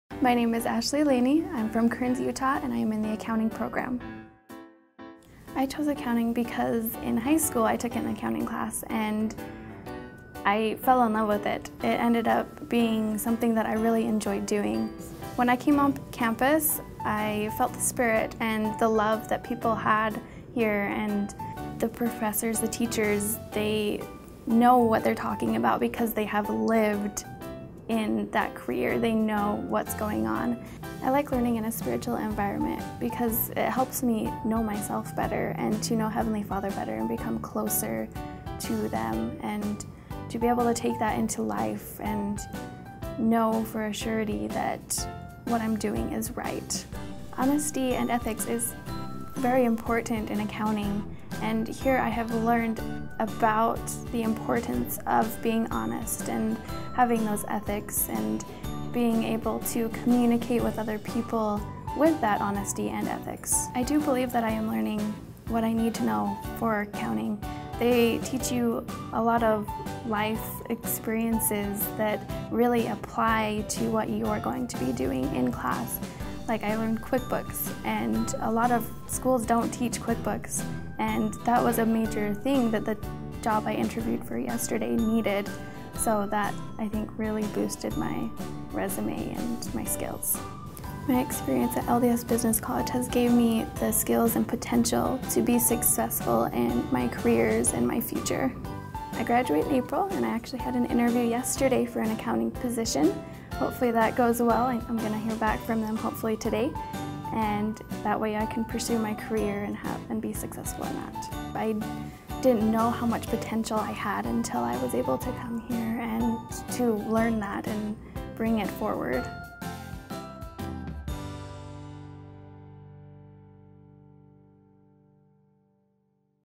Skills and Potential - Ashley Leany at LDSBC - Accounting
Ashley Leany always knew she wanted to go into Accounting and thanks to LDS Business College, she has the skills to get the job she wants.